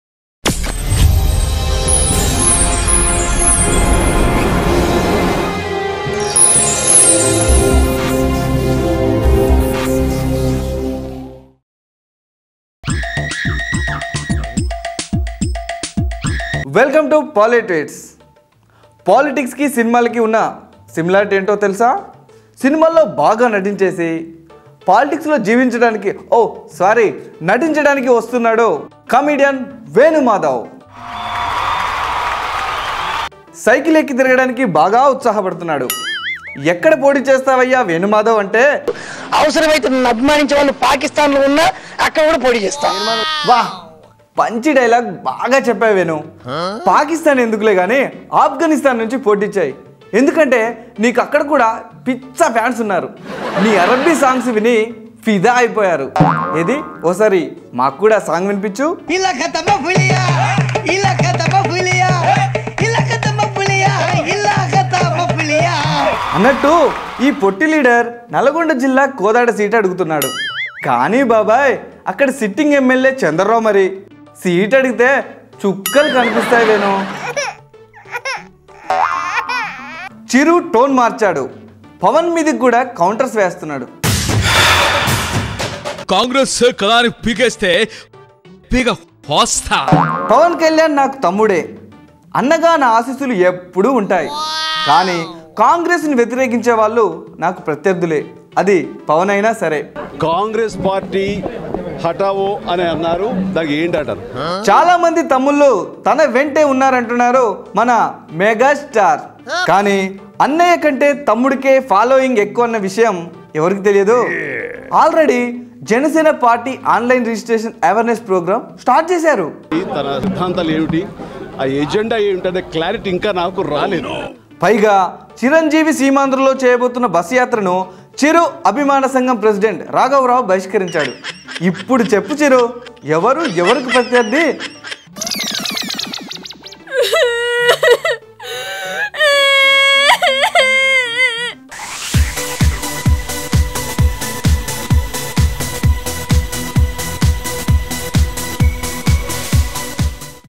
Poli Tweets || Venu Madhav to Contest on TDP Ticket ?
Satirical News Bulletin On Latest Happenings In Indian Politics